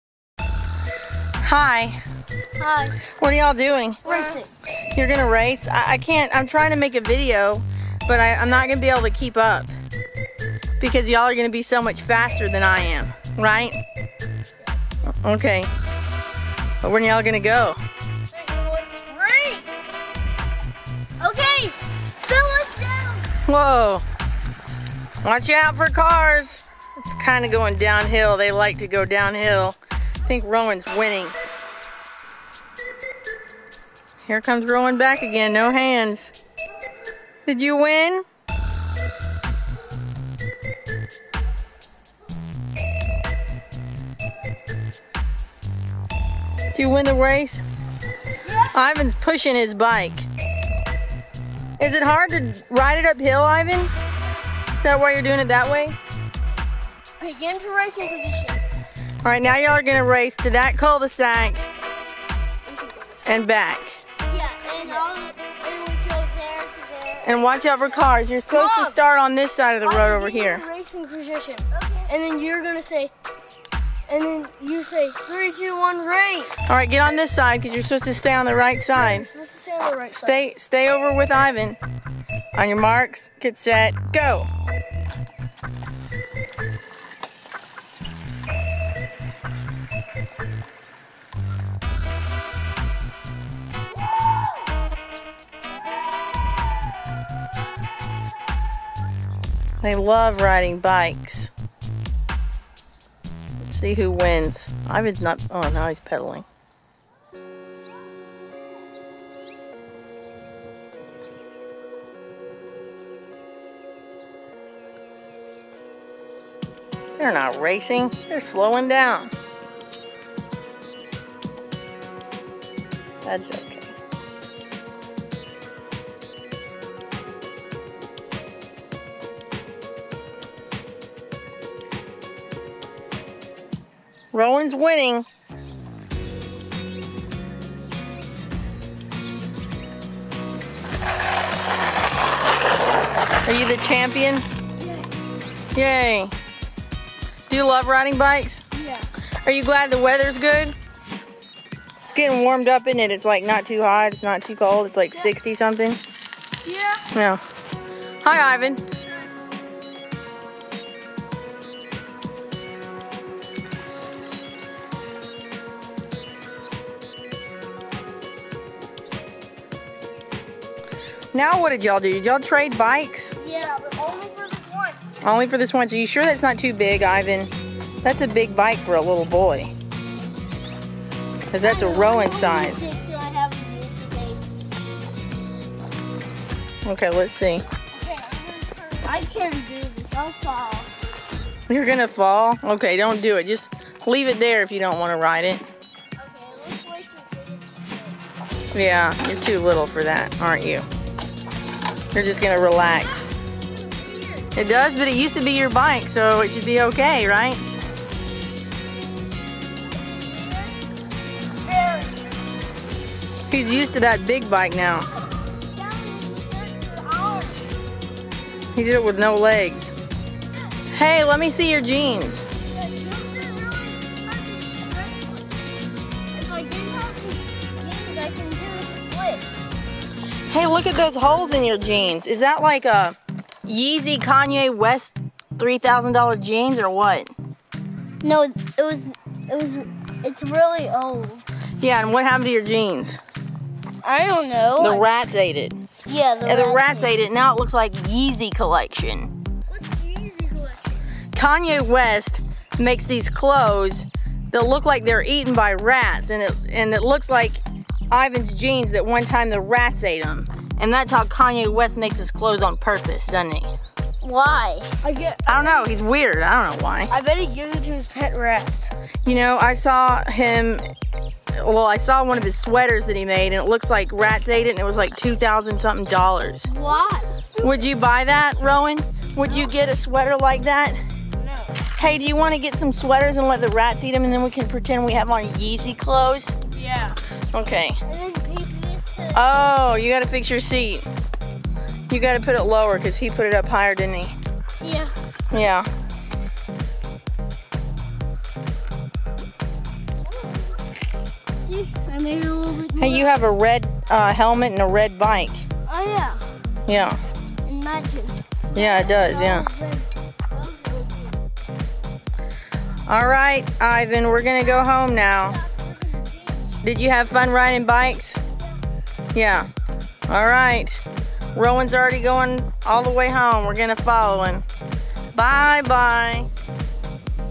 Racing bikes
Rowan and Ivan race on their bikes.